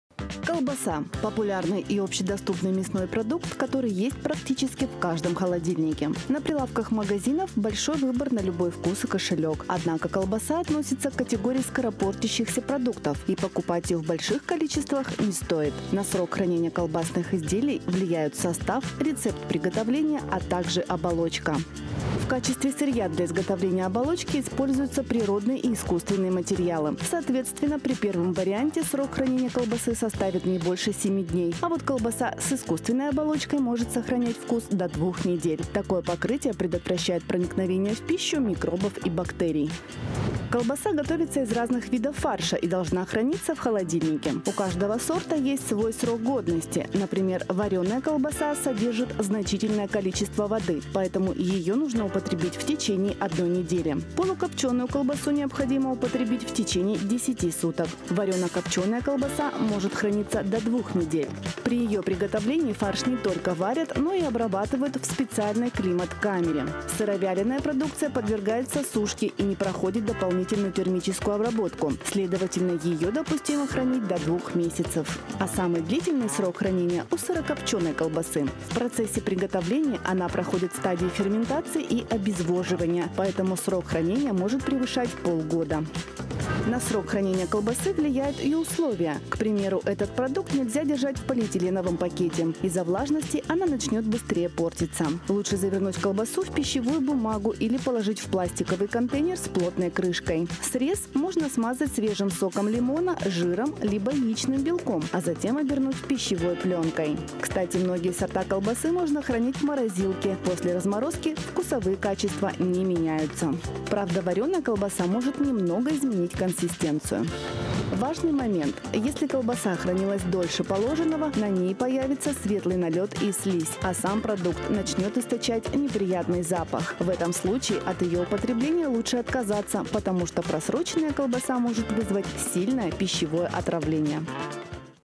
Срок хранения колбасы в рубрике «Срок годности»
Колбаса — один из самых популярных продуктов питания. В качестве основного ингредиента при его производстве выступает фарш в оболочке. Поскольку он является скоропортящимся продуктом, срок годности колбасных изделий небольшой. Но как правило, эту дату производитель на упаковке не пишет. Мы покупаем колбасу, сосиски и прочие виды колбасы и храним её в холодильнике, пока не съедим. На самом же деле, у колбасы, как и других продуктов, есть срок, за который ее необходимо употребить. Подробнее в рубрике «Срок годности».